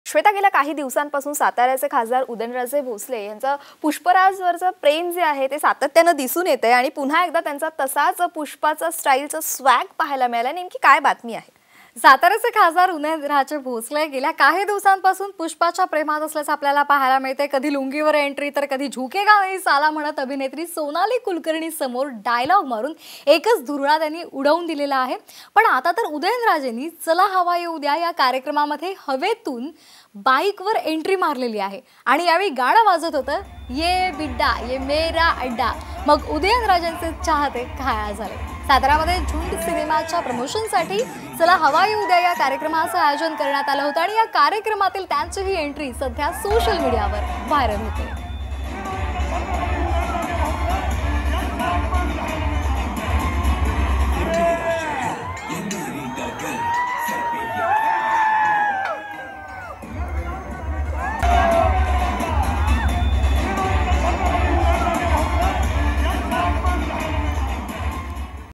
Udayan Raje Bhosle : उदयनराजेंचा व्हिडीओ तुफान व्हायरल,उदयनराजेंची ही हटकी पुष्पा स्टाईल पाहिलीत का?
Udayan Raje Bhosle: Udayan Raje's Video Toofan Viral, Have you seen Udayan Raje's stubborn Pushpa style?